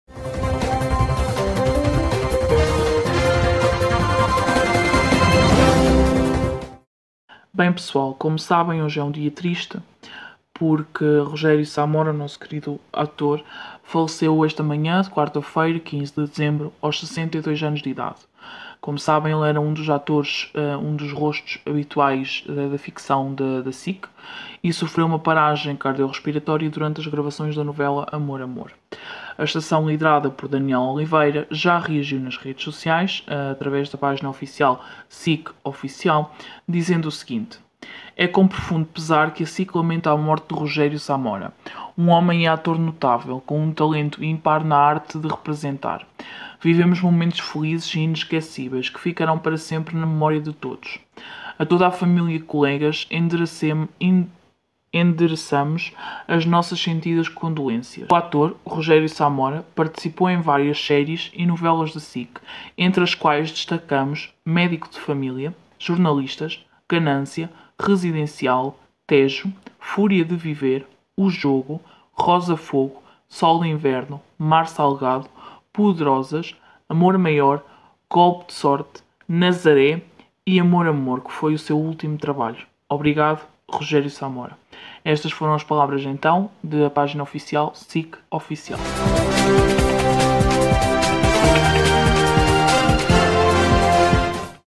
SIC reage à morte de Rogério Samora!
Nos últimos anos, o ator tem participado em várias produções da SIC, como "Nazaré", "Golpe de Sorte", "Amor Maior", "Poderosas", "Mar Salgado" ou "Sol de Inverno".
Nascido em Lisboa a 28 de outubro de 1958, Rogério Samora estudou teatro no Conservatório Nacional, no final da década de 1970, e estreou-se profissionalmente na Casa da Comédia, com a peça "A Paixão". A sua interpretação valeu-lhe o Prémio de Ator Revelação em 1981.
O ator tem uma longa carreira no cinema em filmes de Manoel de Oliveira ("Os Canibais" e "Party"), de Fernando Lopes ("Matar Saudades", "O Delfim", "98 Octanas" e "Os Sorrisos do Destino"), de Luís Filipe Rocha ("Sinais de Fogo"), João Mário Grilo ("Longe da Vista"), de António Pedro Vasconcelos ("Os Imortais"), de João Botelho ("O Fatalista" e "A Corte do Norte") ou de José Fonseca e Costa ("Viúva Rica, Solteira Não Fica"), entre outros.
Antes de se mudar para a estação de Carnaxide, Rogério Samora protagonizou várias novelas e série da TVI, como "Destino Imortal", "Flor do Mar", "Equador", "Casos da Vida" e "Fascínios".
Na televisão, o ator fez parte ainda do elenco de "Jura" (SIC), "O Jogo" (SIC), "Super Pai" (TVI), "Médico de Família" (SIC), "A Mulher do Senhor Ministro" (RTP1) ou de "Os Melhores Anos" (RTP1). "Vila Faia", em 1982, marcou a estreia de Rogério Samora no pequeno ecrã.
Atualmente o ator estava a gravar a segunda temporada de 'Amor Amor' quando se sente mal. 5 meses depois, a 15 de Dezembro de 2021 acaba por falecer.